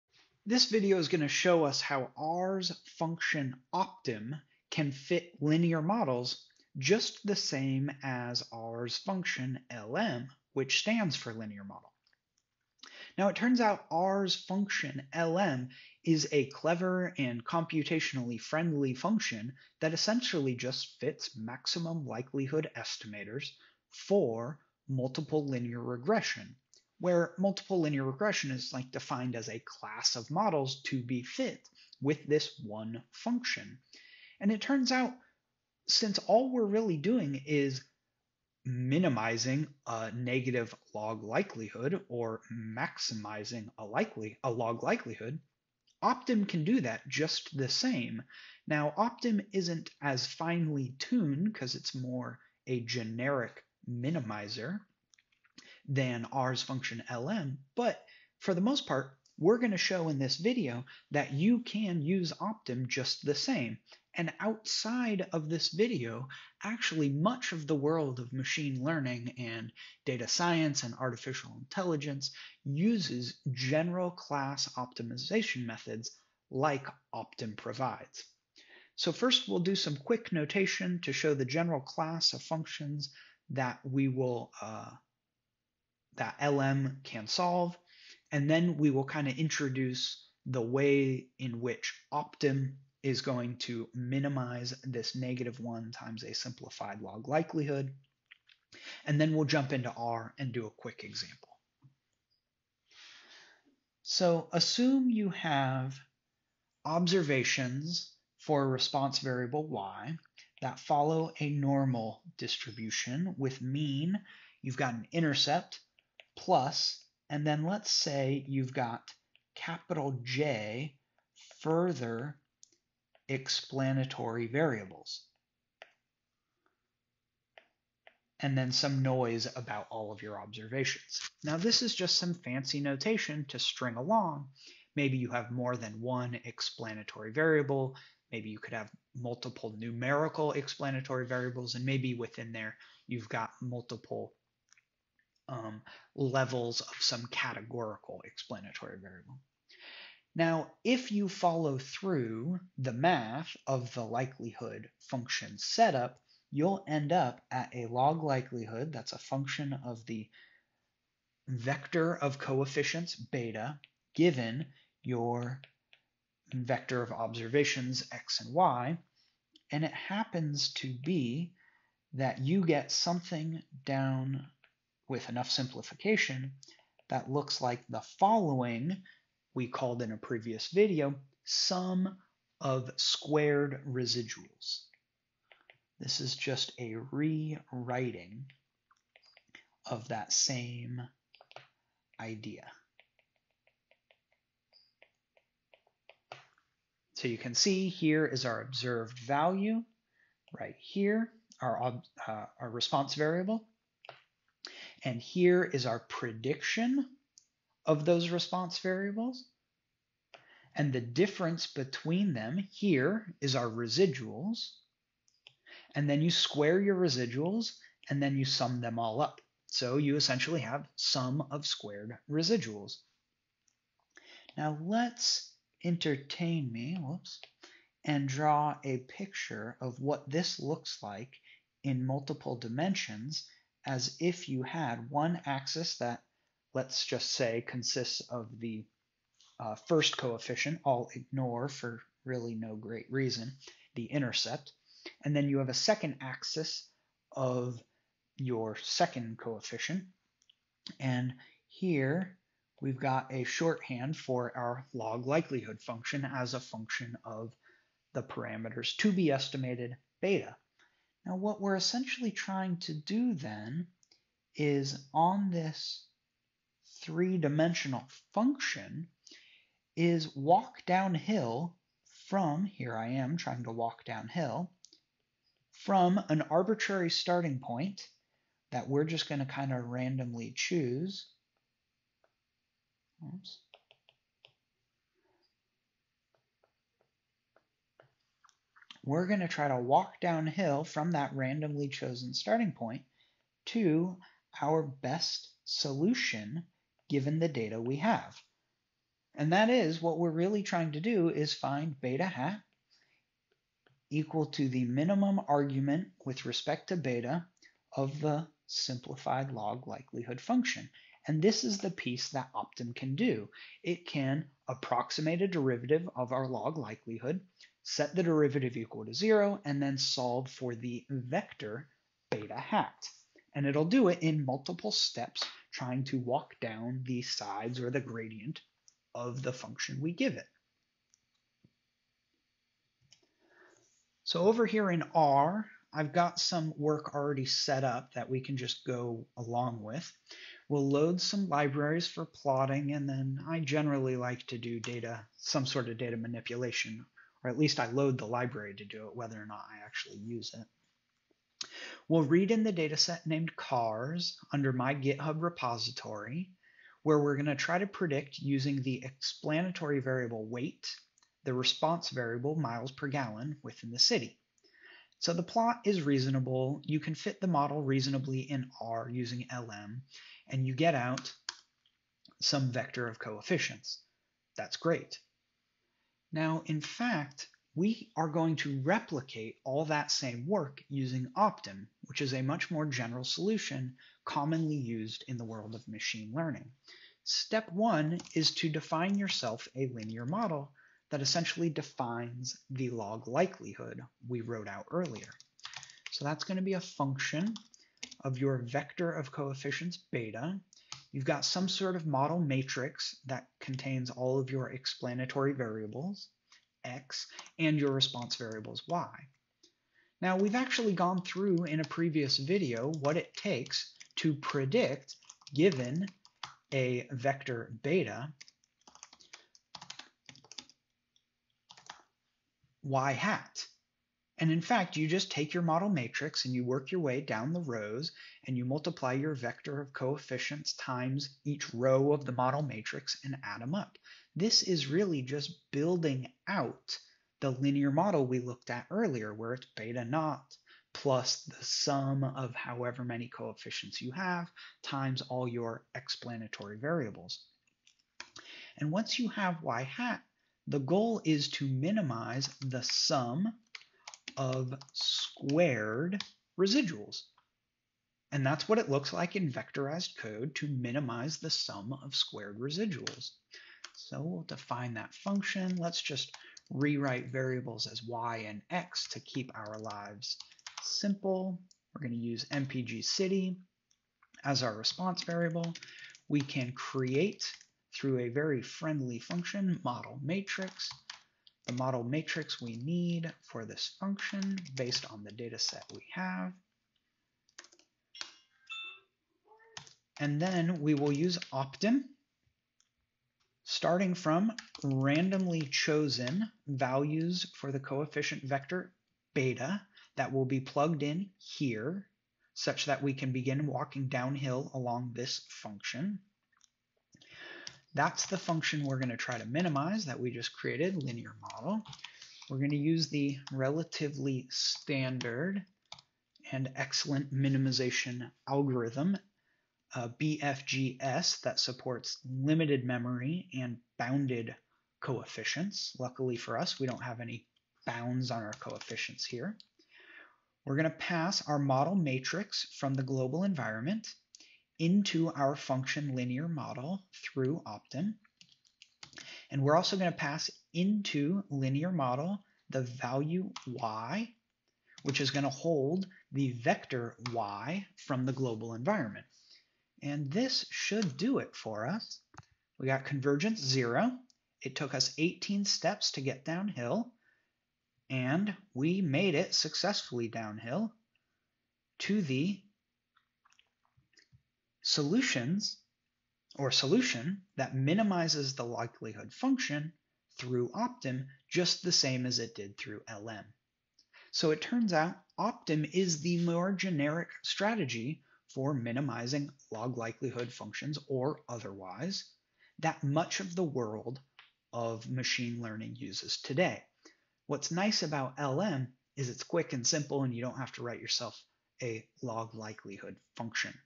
A Short Introduction to Fitting Simple Linear Regression Using R's optim()
A 10 minute introduction to using R's optim() to fit simple linear regression.